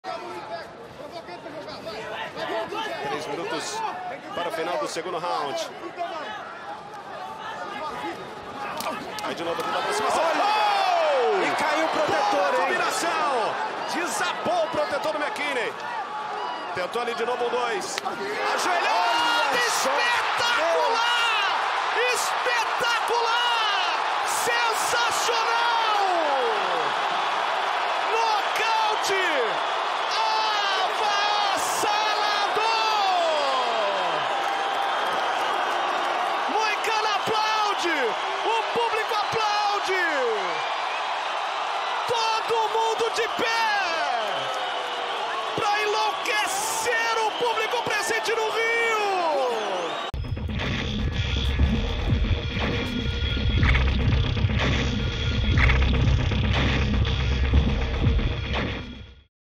Ismael "Marreta" Bonfim nocauteia Terrance McKinney com uma joelhada voadora | UFC 283
Confira o nocaute do brasileiro Ismael "Marreta" Bonfim sobre Terrance McKinney em duelo pelo peso-leve no UFC283.